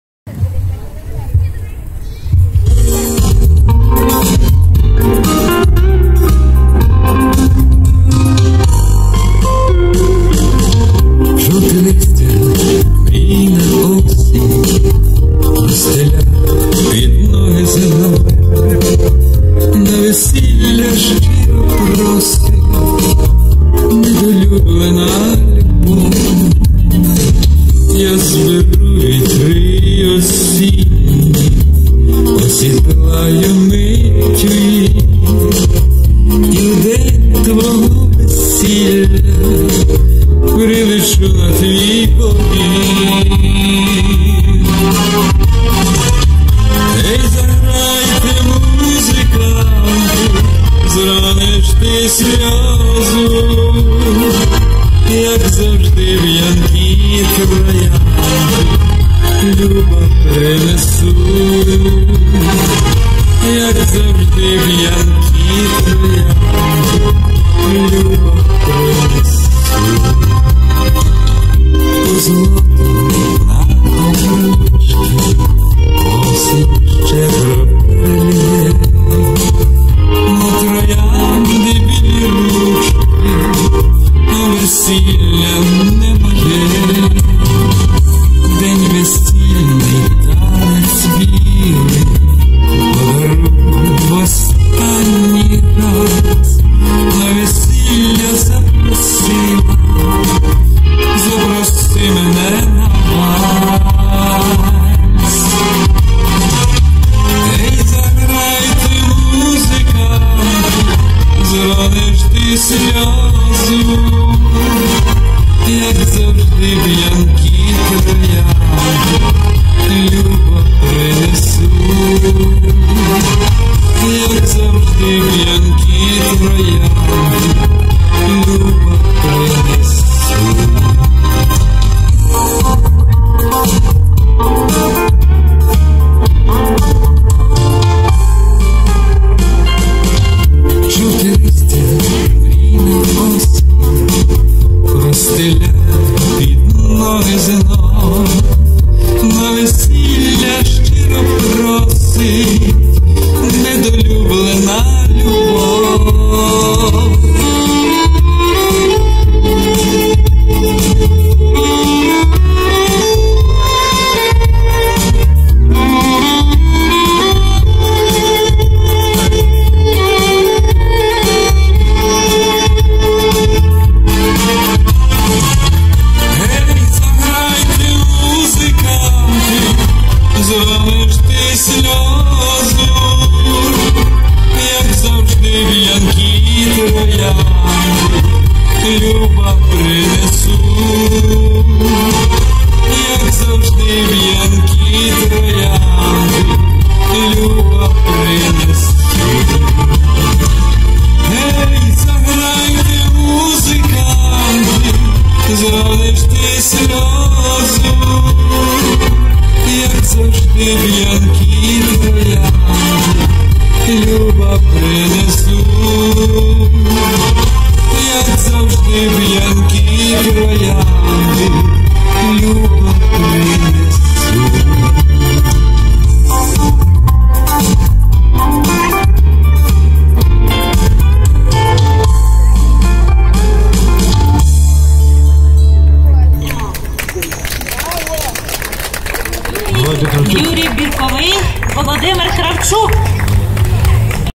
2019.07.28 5-й вечір вуличної музики Збараж, організований Збаразька міська рада за підтримки Збараж-БУР.